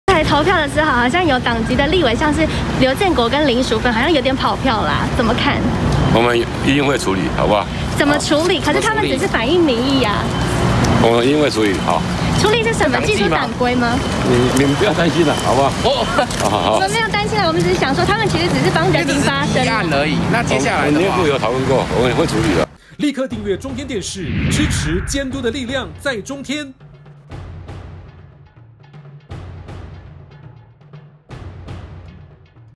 20201224中天新聞 表決大戰綠委跑票 柯建銘：一定會處理
表決大戰綠委跑票，柯建銘：一定會處理。